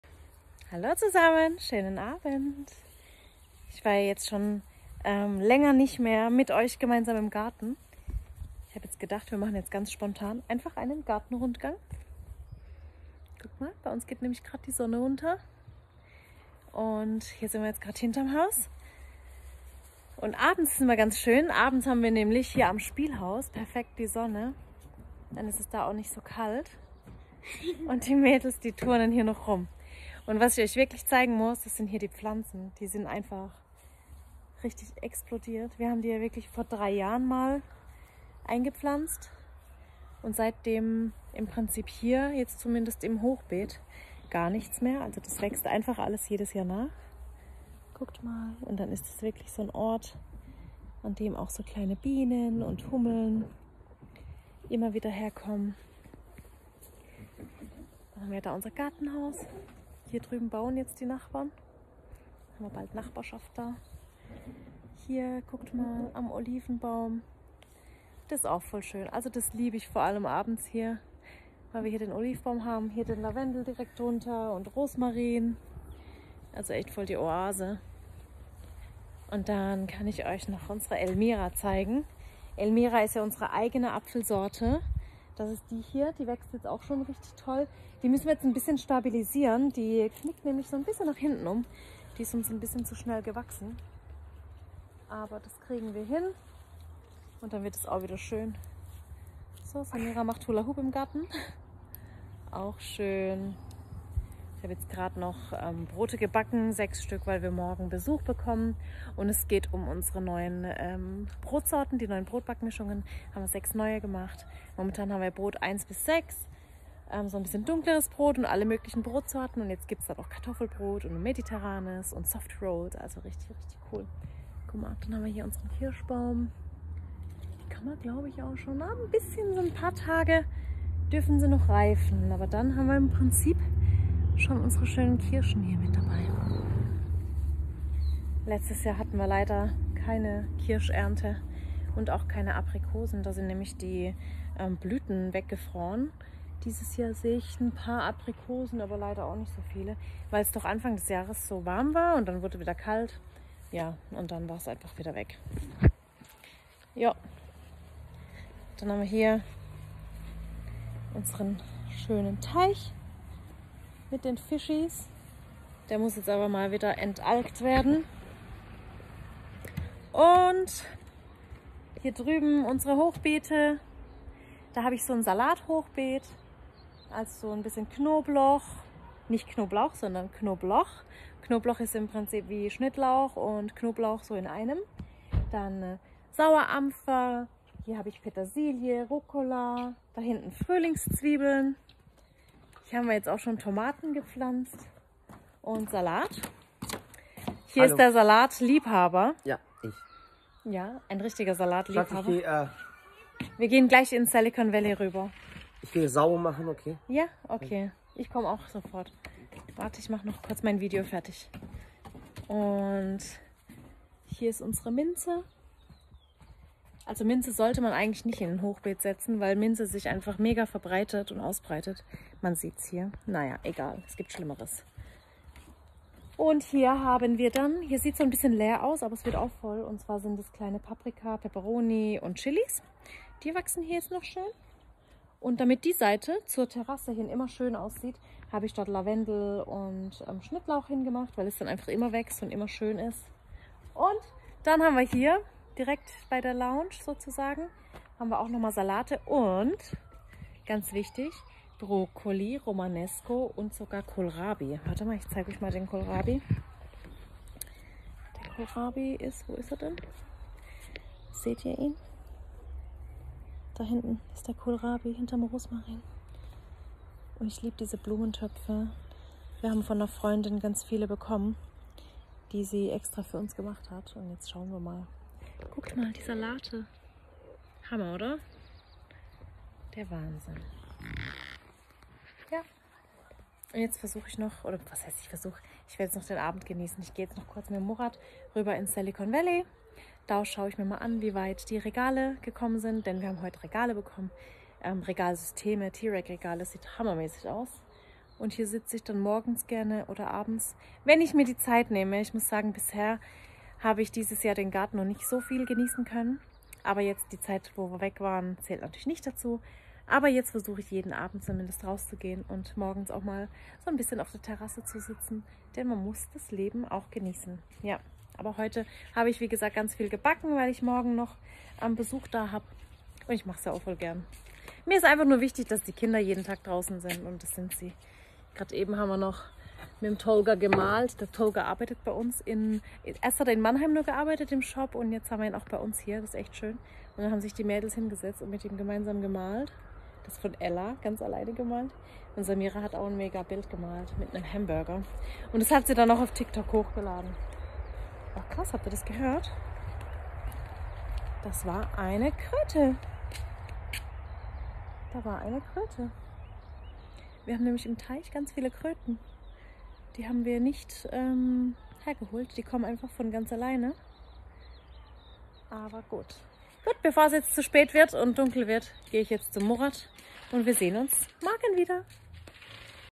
Garten Tour / Garten Rundgang
Ich zeige euch in meinem mal unseren Garten. Ein Rundgang rund um das Haus herum. Alles wächst so schön gerade! Ich liebe die Natur und unseren Garten 🪴 Datenschutz